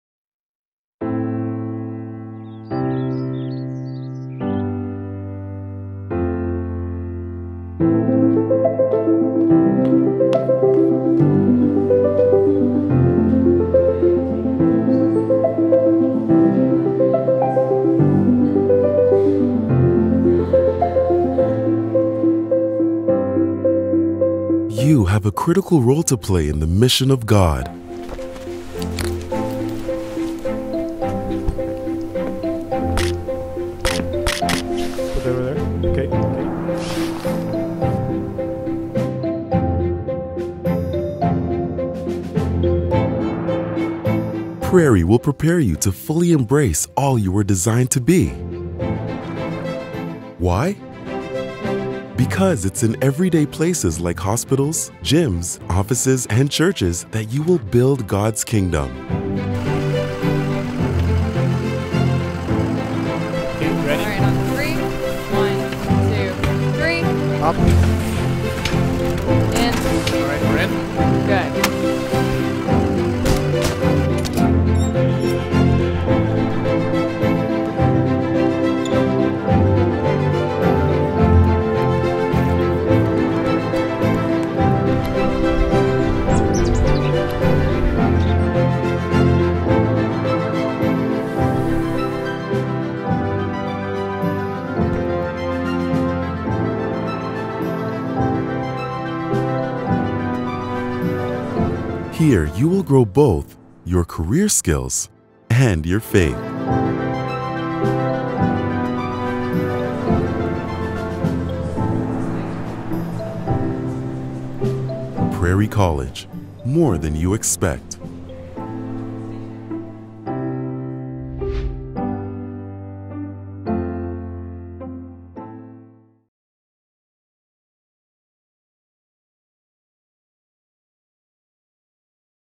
Prairie College Promo Video
Prairie College is a private, interdenominational Christian college in Three Hills, Alberta. With a commitment to academic excellence and spiritual discipleship, combined with a genuine and welcoming community, the school offers a formative life-changing experience.

With 300 students from all corners of the globe, 30 programs, and a network of 17,000 alumni, this dynamic campus community is globally-minded and driven to meet the greatest needs of the world.